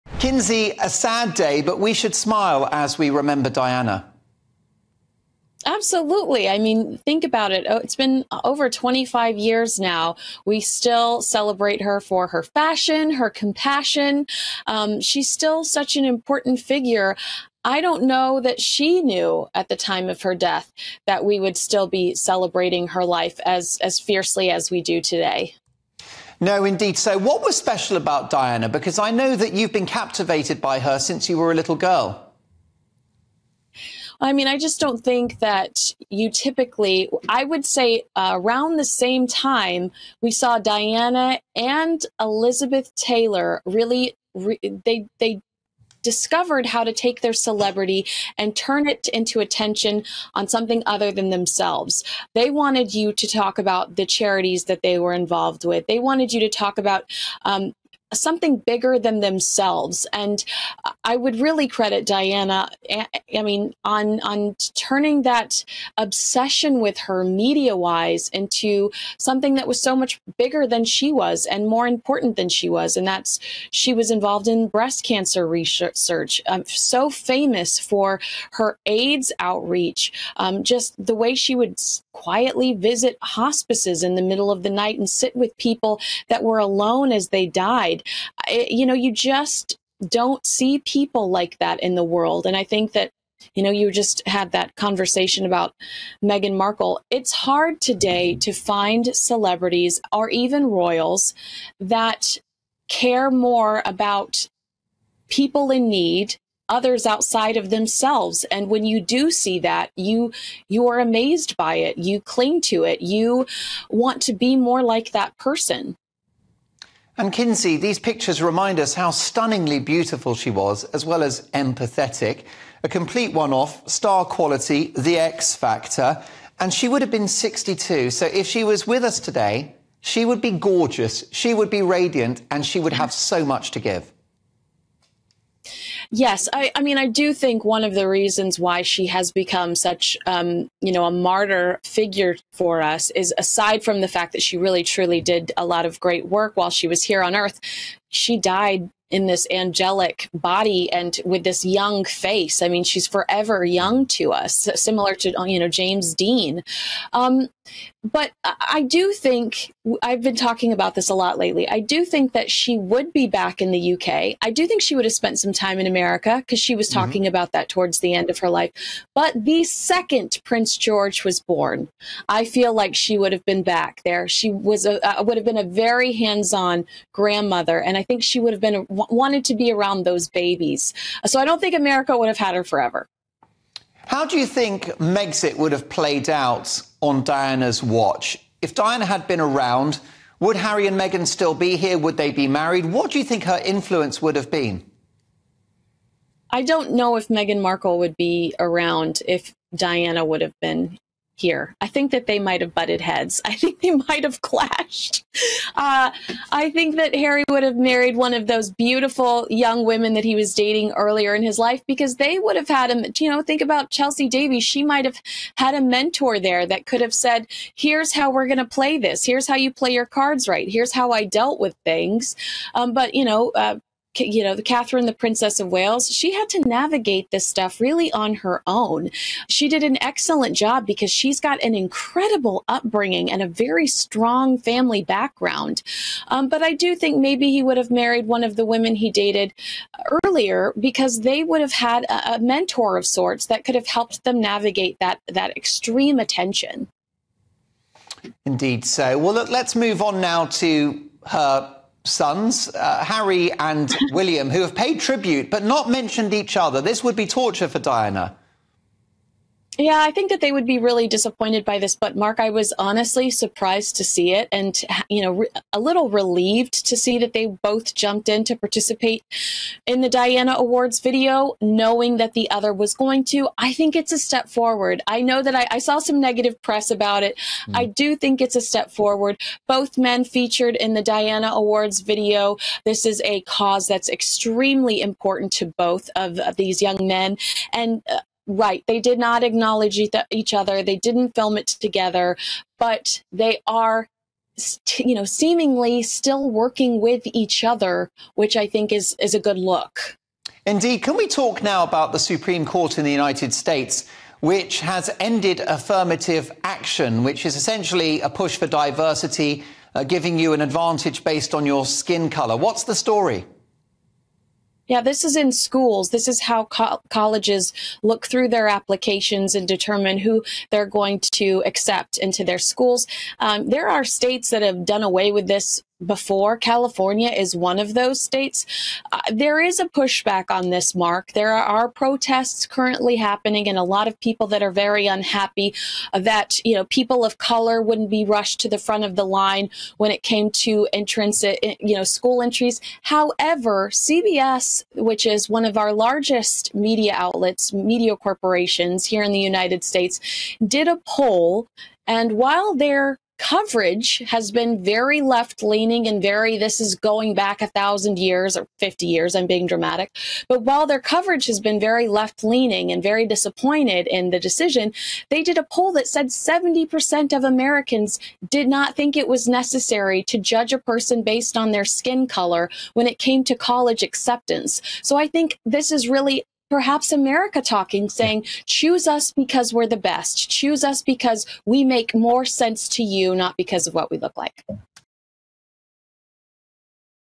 Meghan Markle 'wouldn't be around if Diana was still here!' | Kinsey Schofield
Kinsey Schofield discusses Princess Diana on her 62nd birthday and her possible relationship with Meghan Markle.